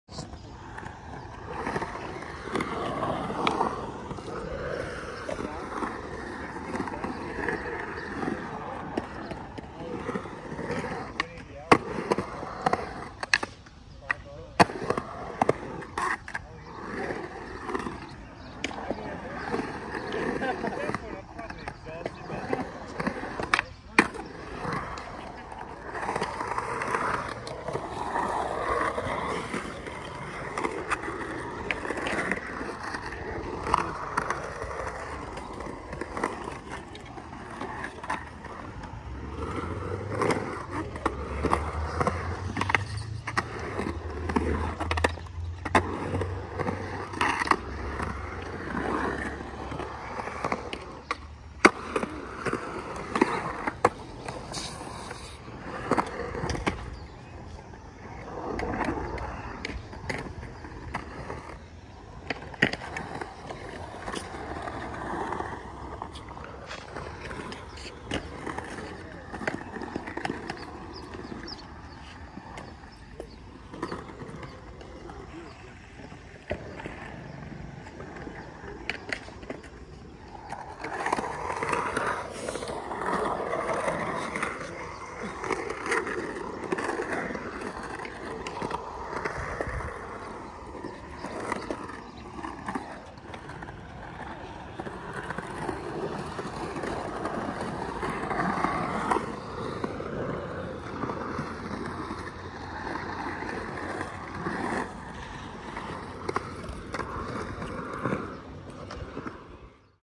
Go Skateboarding Day 2020 - Sayreville Skatepark
Brian Grande, Robin Grande and Bill Garolis skate Sayreville Skatepark on Go Skateboarding Day 2020.